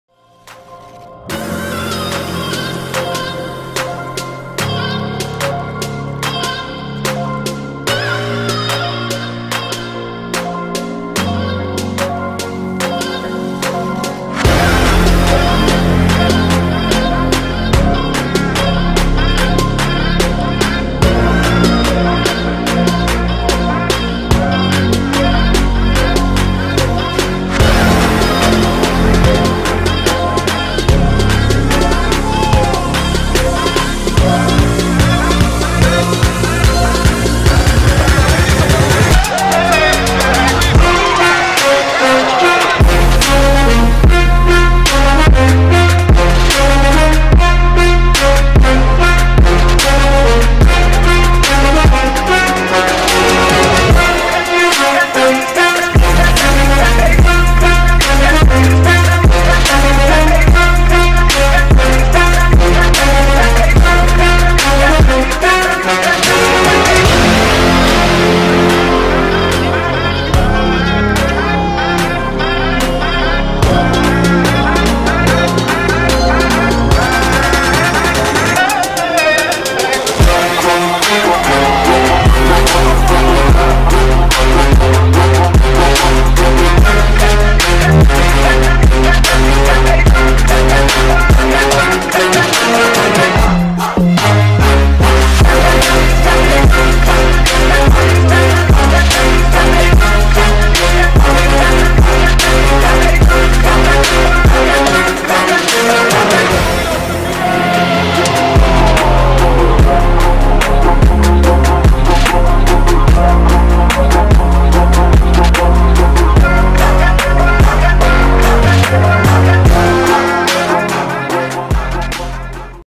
️Rotabet Yıldızlar Geçidi #72 | Güven Yalçın
⭐️Rotabet Yıldızlar Geçidi #72⭐️

Bahisin Değişmez Yıldızı Rotabet'te Bu Haftanın Konuğu;

Beşiktaş'da 3 Sezonda Çıktığı 101 Maçta Attığı 20 Gol, Yaptığı 7 Asist ve Kaldırdığı 3 Kupa ile

Güven Yalçın! 🇹🇷 ⠀